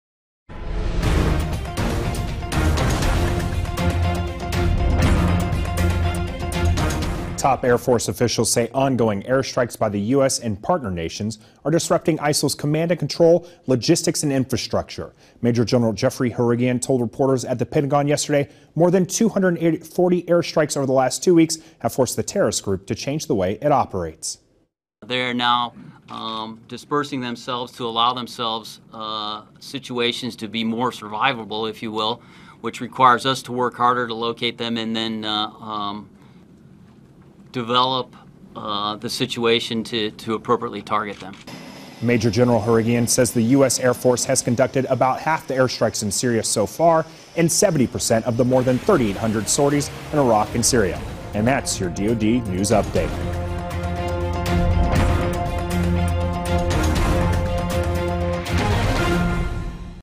Airstrikes Dispersing ISIL
09/30/2014
The Pentagon, District of Columbia, United States
℠2014 - Air Force Maj Gen Jeffery Harrigian says airstrikes are forcing ISIL to change the way it operates.

US DoD